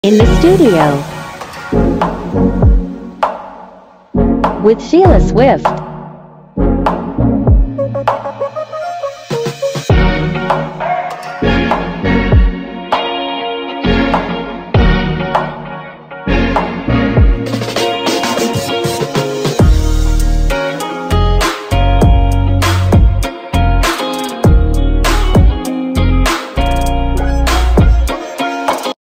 In The Studio w Sheila Swift Kahé
WHAT ARE YOU creating?!?
Here are some of my new ideas! Love making a mess and cutting through the chaos when my brain tries to figure out what shapes and colors are more fun 😝🦄💯🕺🏽☀️🎨